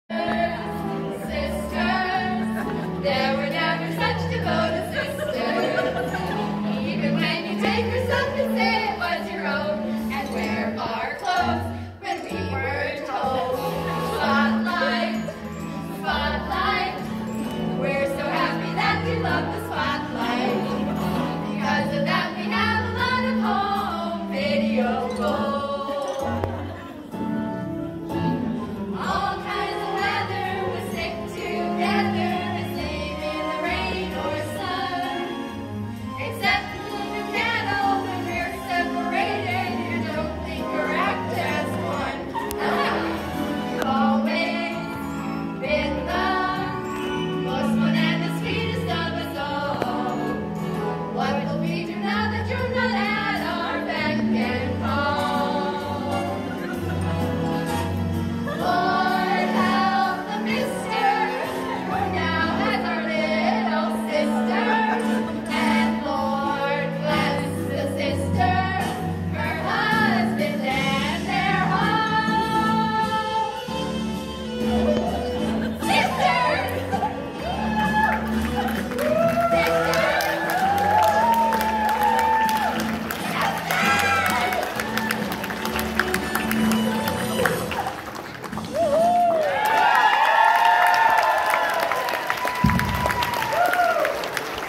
Caleb & Lydia's Funny maid of honor Wedding Speech Song
4 Bridesmaids singing best remixed song to Bride and Groom ,

sister speech best friend